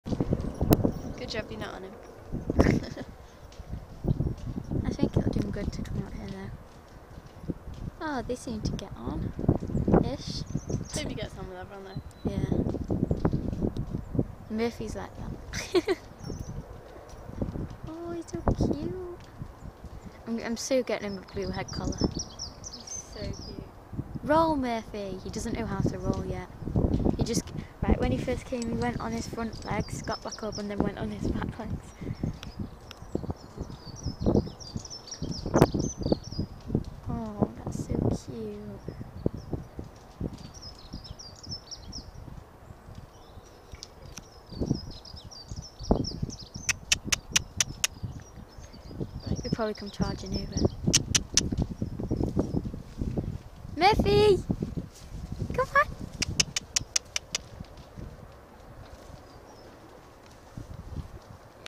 murphy has made a friend !
soo murphy has made a new friend called toby BFFL xx